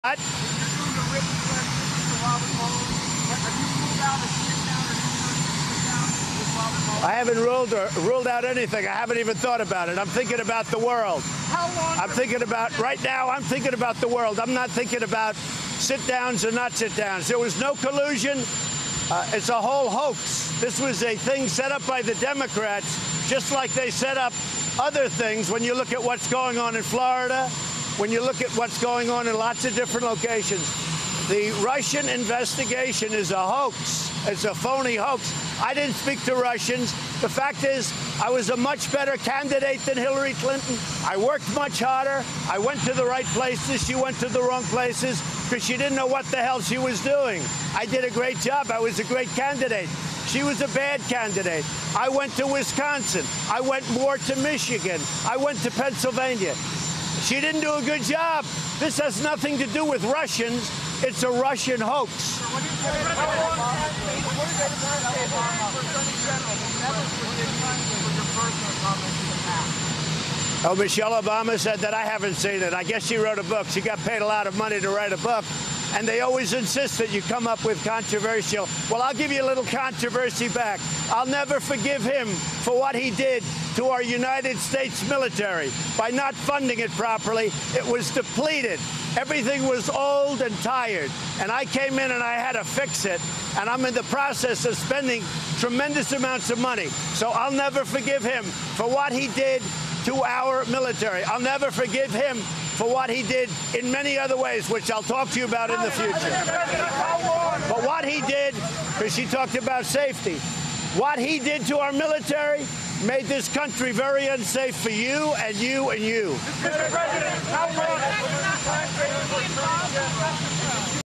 Trump on the world, Russians, Hillary’s 2016 election and Michelle Obama’s book
Trump talks to the media (9 Nov 2018)  1 / 6